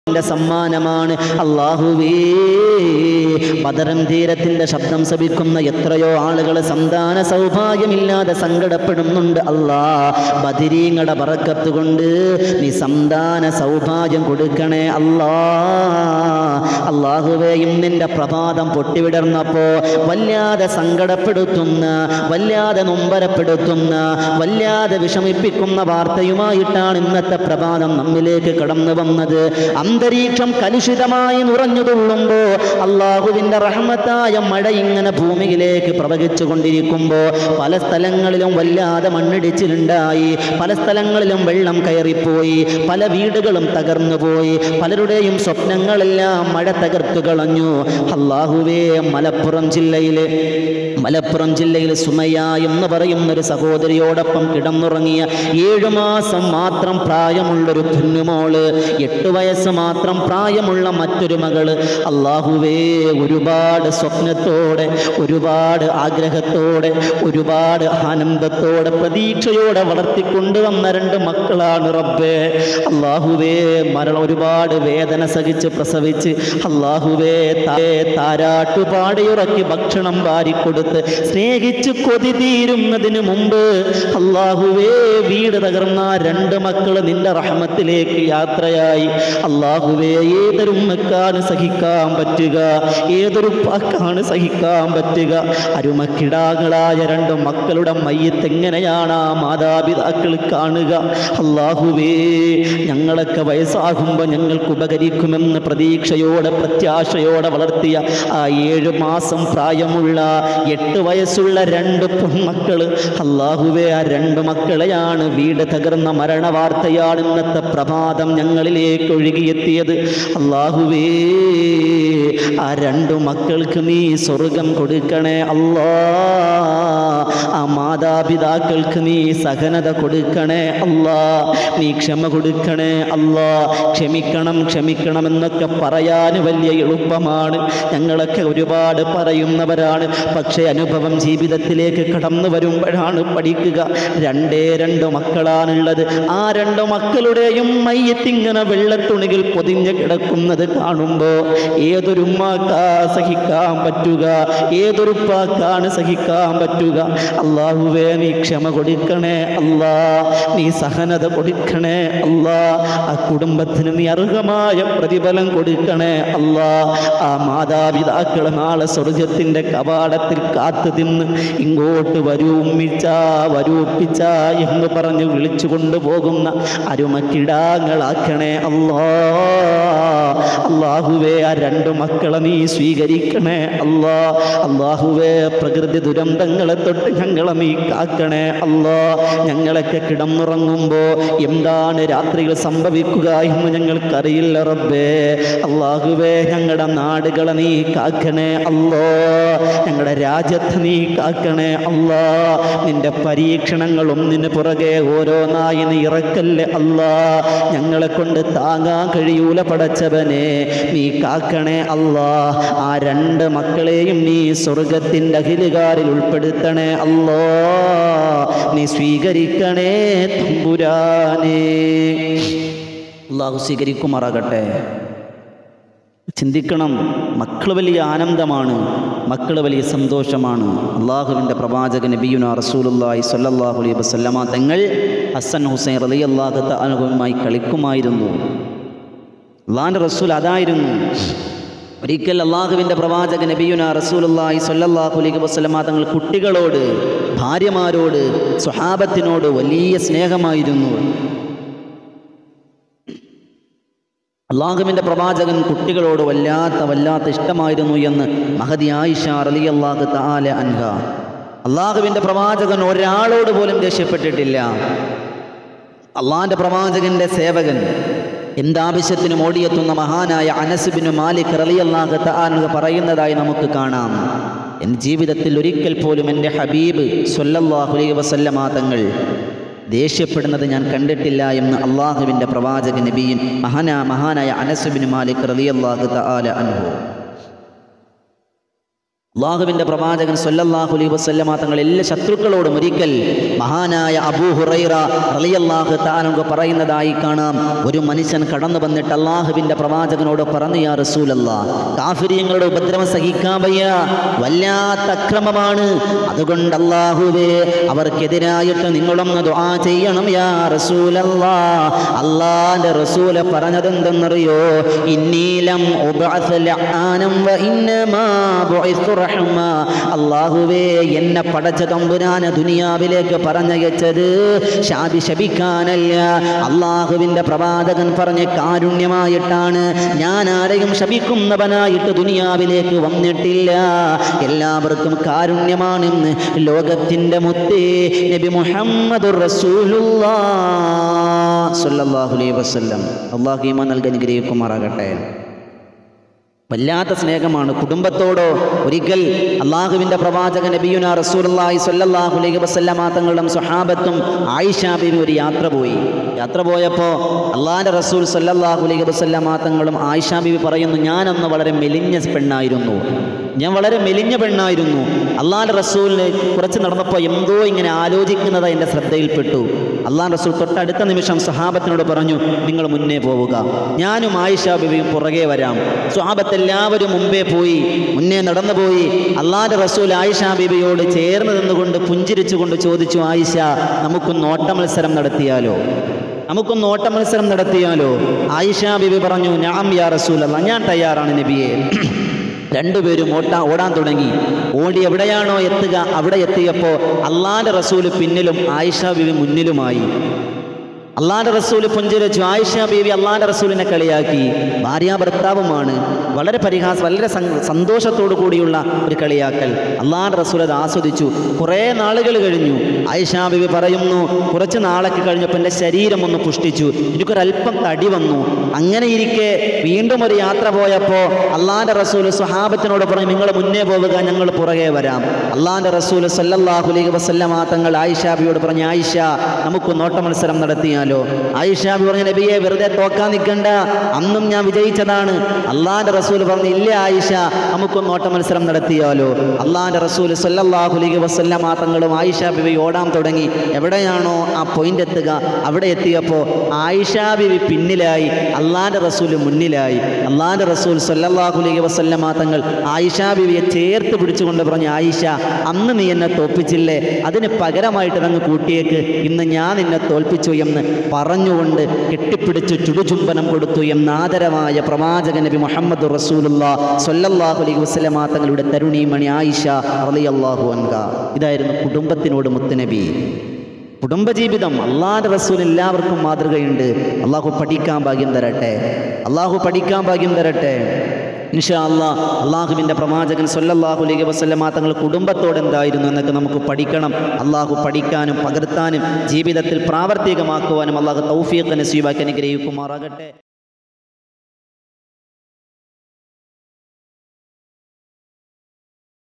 മലപ്പുറത്ത് വീടിടിഞ്ഞു മരിച്ച പൊന്നോമനകൾക്ക് ബദറിൻ തീരത്തിൽ ബദരി ഉസ്താദ് പൊട്ടിക്കരഞ്ഞു ദുആ
മലപ്പുറത്ത് വീടിടിഞ്ഞു മരിച്ച പൊന്നോമനകൾക്ക് ബദറിൻ തീരത്തിൽ ബദ് രി ഉസ്താദ് പൊട്ടിക്കരഞ്ഞു ദുആ ചെയ്യുന്നു